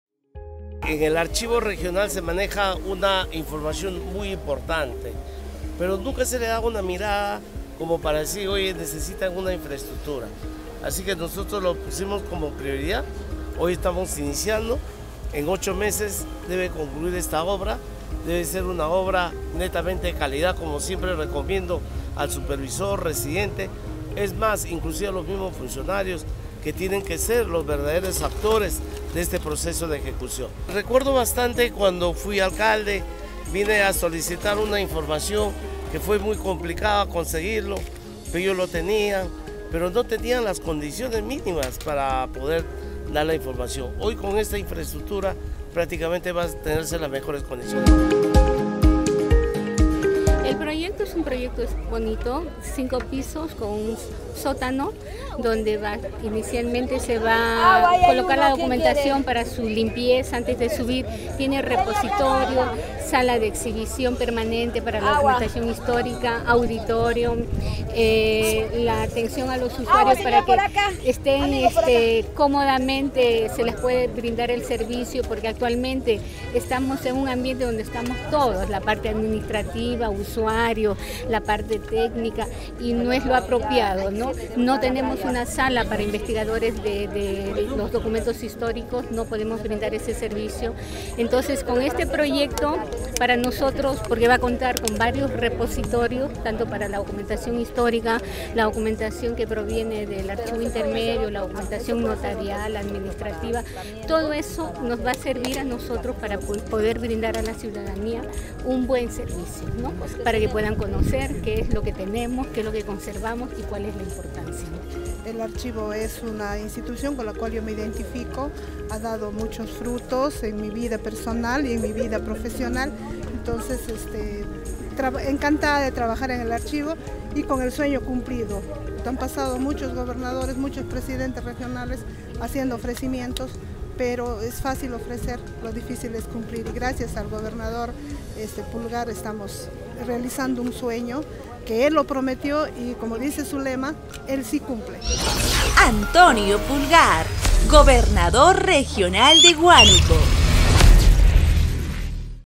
🔰🔱 INICIAMOS CONSTRUCCIÓN MODERNO ARCHIVO REGIONAL DE HUÁNUCO
💪 Después de 40 años, la Dirección Regional de Archivo de Huánuco tendrá una moderna infraestructura de cinco niveles, que permitirá custodiar el patrimonio cultural y documentario con una tecnología de última generación.
La obra, que será ejecutada por el Gobierno Regional Huánuco (Gorehco), con una inversión de 6 millones 171 soles, se desarrollará en un área de 310 metros cuadrados, contará con un semisótano, ascensor y sistema de motocarga. Asimismo, será equipada con mobiliarios y equipos para el almacenamiento digital de archivos, áreas de recepción y administrativo. Igualmente, se adquirirá vehículos para el transporte de archivos regionales ubicados en diferentes puntos de la ciudad.